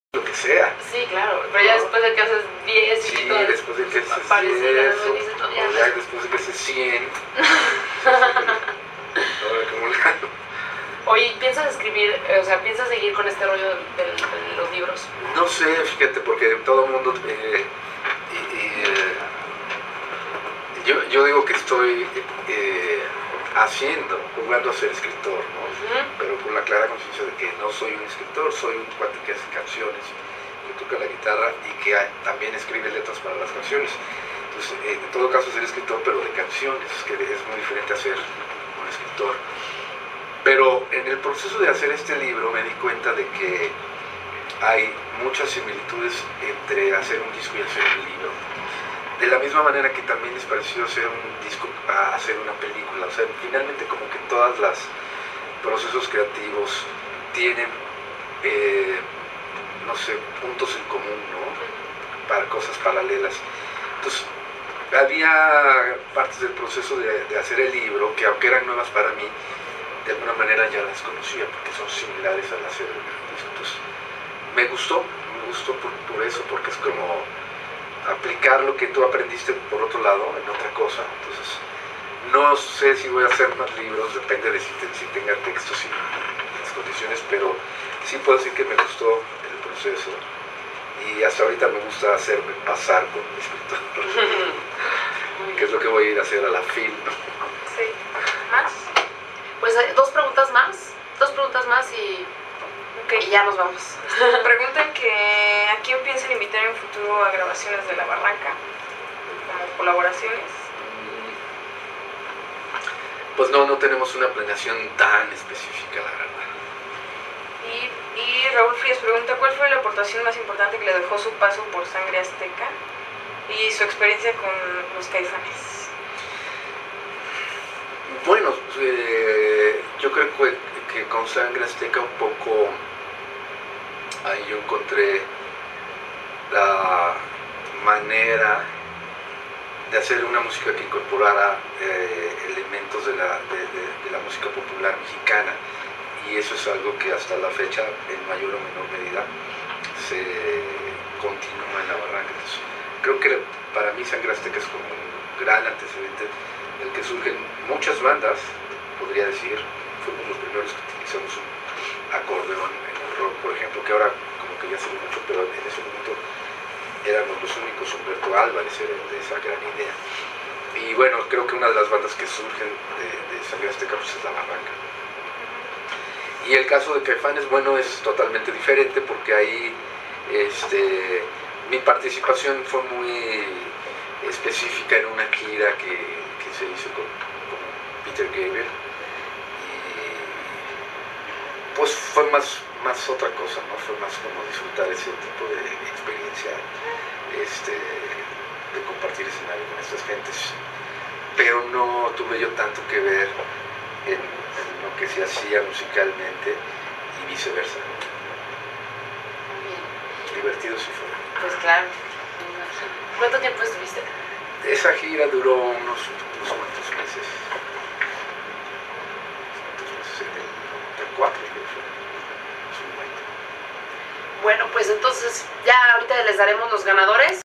Jose Manuel Aguilera en entrevista
Fragmento de una entrevista con Jose Manuel Aguilera hablando de su nuevo libro "Estambul", de su experiencia con "Sangre Azteka" y "Caifanes"